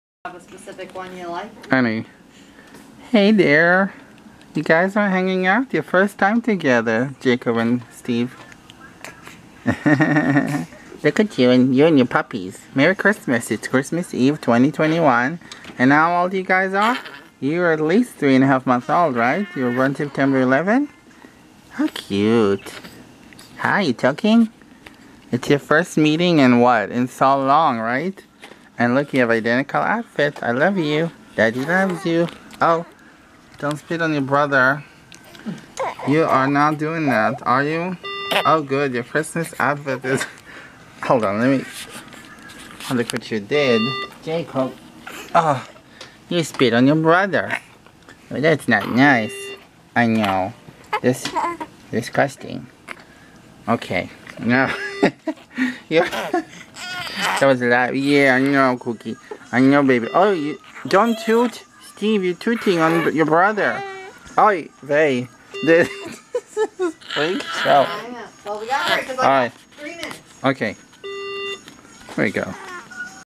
Merry Christmas 🎅 and Happy New Year 2022 🎇 from Papa 🐶 Puppy and the Twins 🍼 Jacob and 🍼 Steve!!!
This shirt clip was made on Christmas 🎅 Eve 2021. On New Year's Eve the twins have spent 111 days at the NICU. We wish everyone a blessed year with lots of 😍 love, joy, and fun!